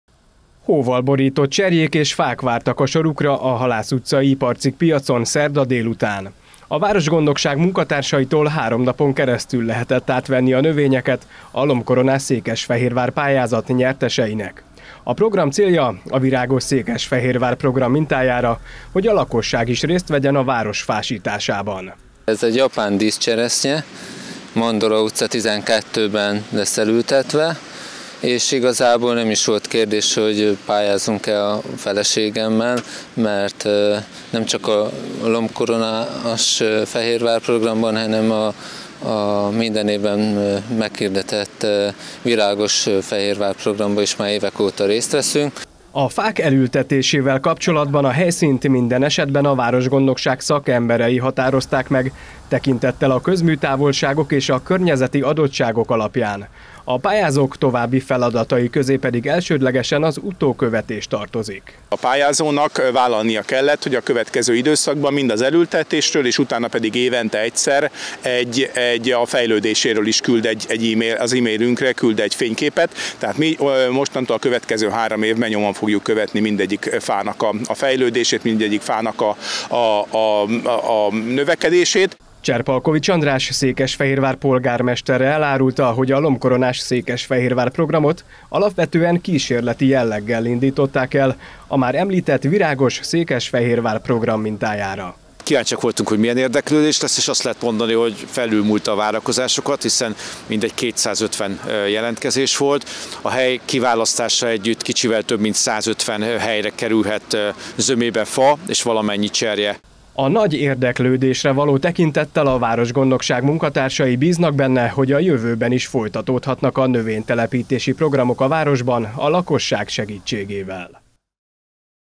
Kiosztották a növényeket
Következő szakaszához érkezett a LOMBkoronás Székesfehérvár program. A pályázaton elnyert fákat és cserjéket keddtől csütörtökig osztják ki a nyerteseknek a városgondnokság munkatársai.